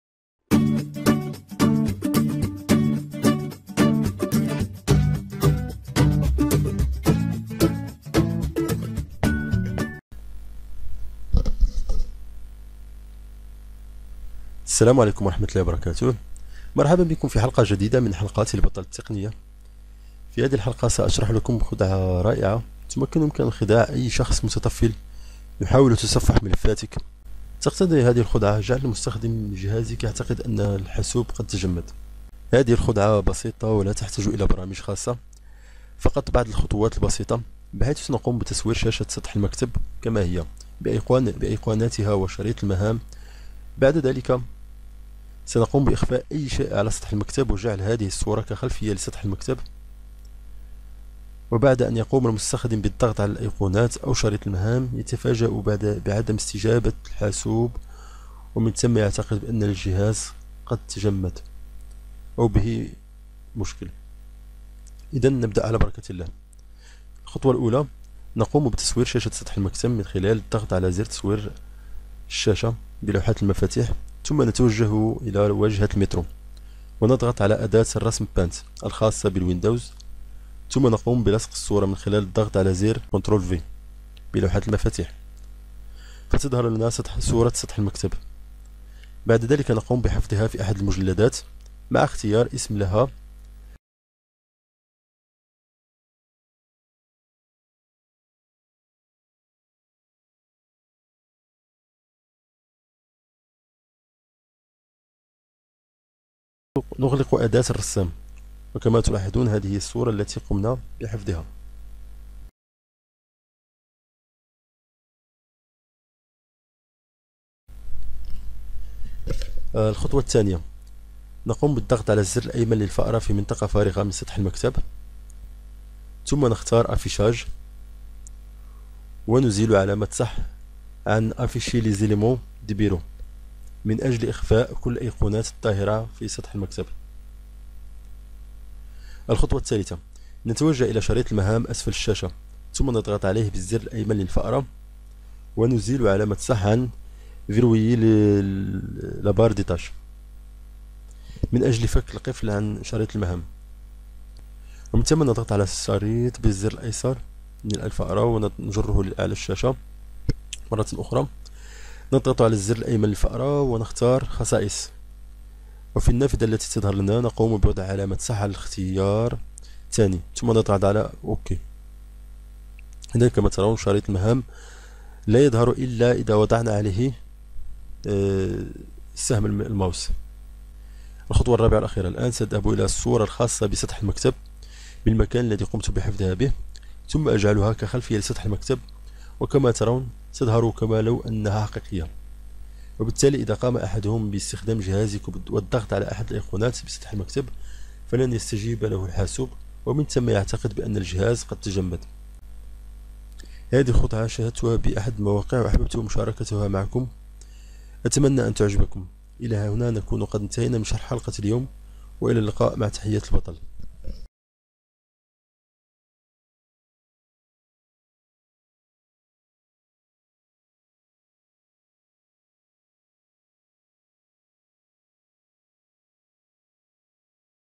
خدعة بسيطة تبهر أي متطفل و تمنعه من استخدام حاسوبك
خدعة لإخغاء سطح المكتب حتى لا يستخدم اي متطفل حاسوبك
رابط الموضوع :